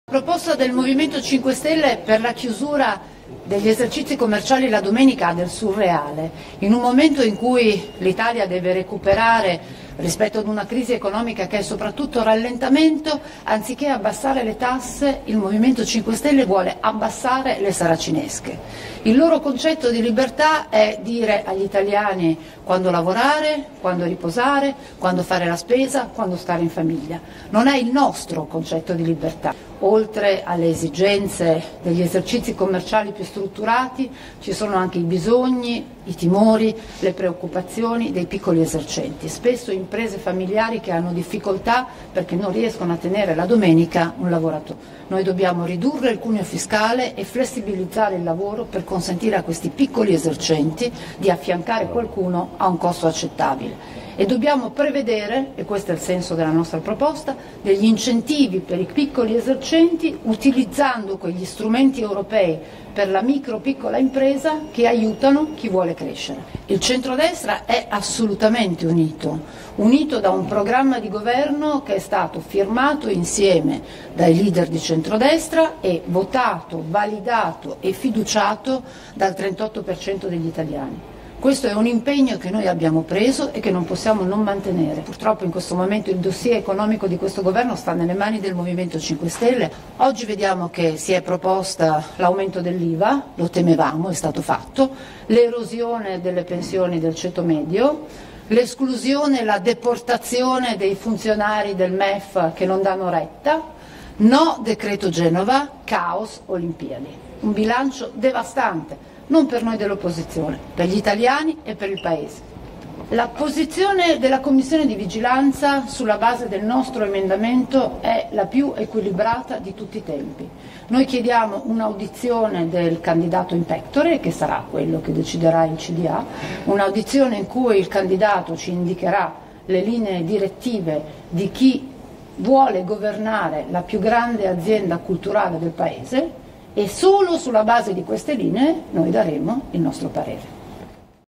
Manovra, Bernini: "M5S prende decisioni surreali e illiberali"
"Abbassano saracinesche, alzano le tasse. Bilancio devastante per italiani e paese"

POLITICA (Roma). L'onorevole Anna Maria Bernini a margine della conferenza sulla chiusura dei centri commerciali di domenica si è soffermata a parlare con i cronisti: "La proposta per la chiusura ha del surreale. Il M5S abbassa le saracinesche e alza le tasse. Il loro concetto di libertà è decidere per gli italiani. La nostra proposta prevede incentivi per i piccoli esercenti utilizzando strumenti europei per aiutare chi vuole crescere. Il centrodestra è unito da un programma di governo firmato dai leader e votato, validato e fiduciato dal 38% degli italiani. Il dossier economico purtroppo è nelle mani del M5S che alza l'Iva, erode le pensioni, crea caos. Un bilancio devastante per gli italiani e il paese. Rai? Chiediamo l'audizione del candidato in pectore in cui il candidato ci indicherà le linee direttive di chi vuole governare la più grande azienda culturale del paese. E sulla base di queste linee noi daremo il parere". (Luigi Pellicone/alanews)